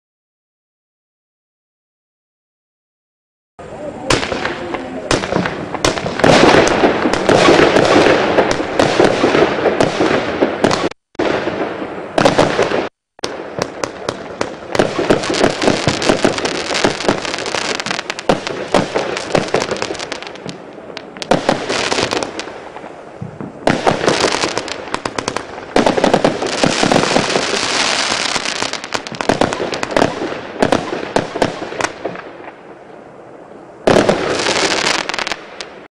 1415 Crazy Nitro Circus (6)
Wisselende schietrichtingen, harde burst, veranderende tempo's en schitterende boeketten!